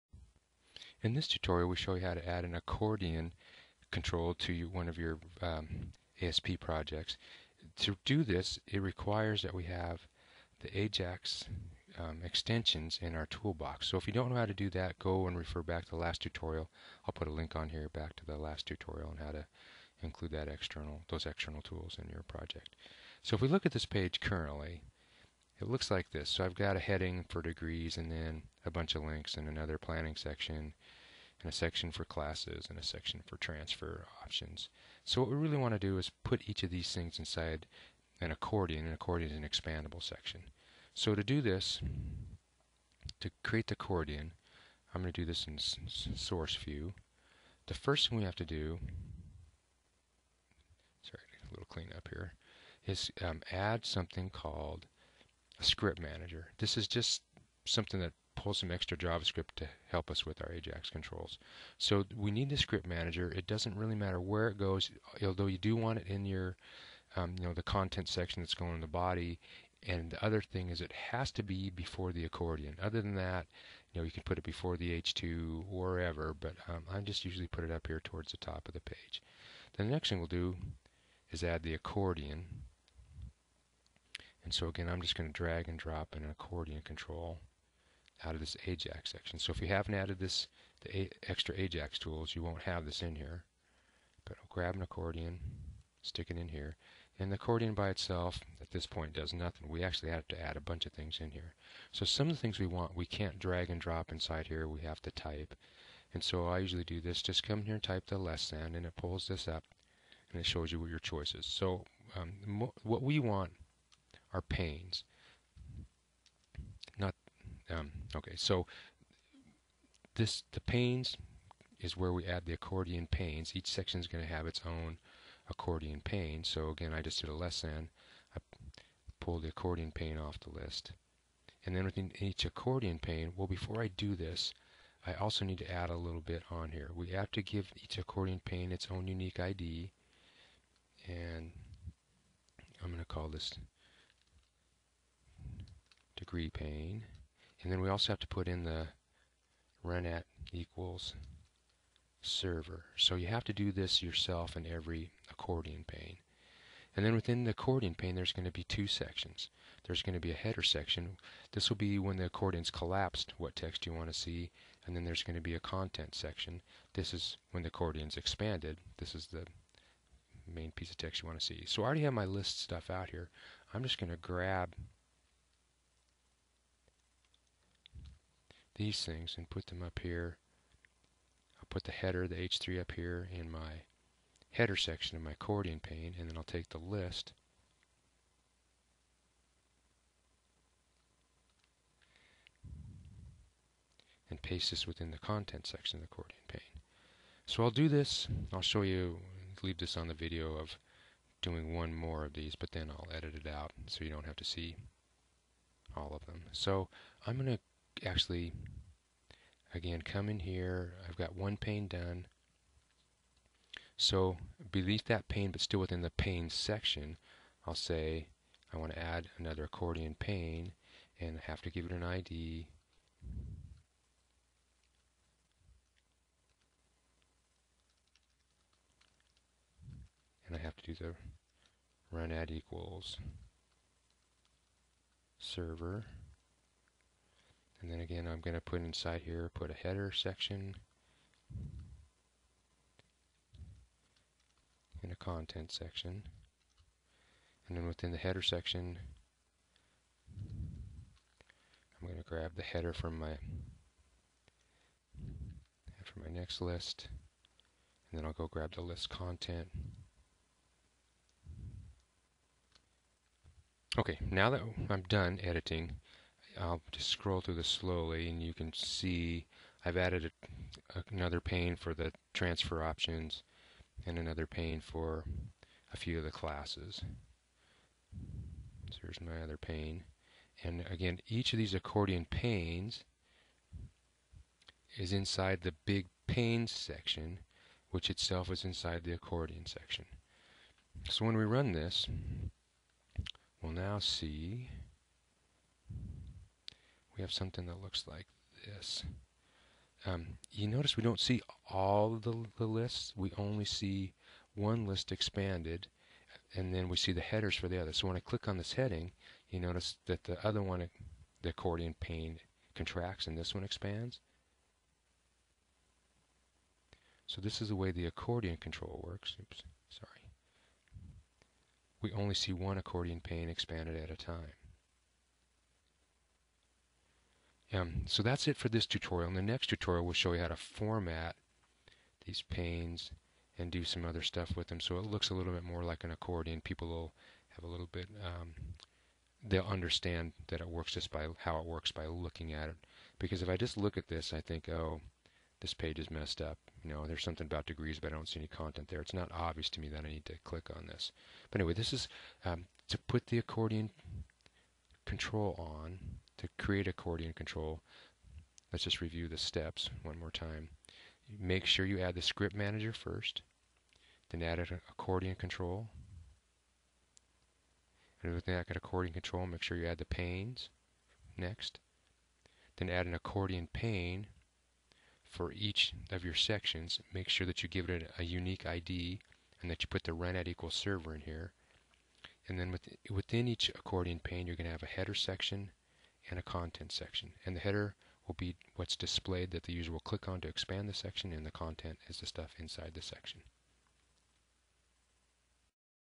asp add accordion.wmv
This video tutorial demonstrates adding an accordion control to an asp.net project. The accordion is one of the ajax controls.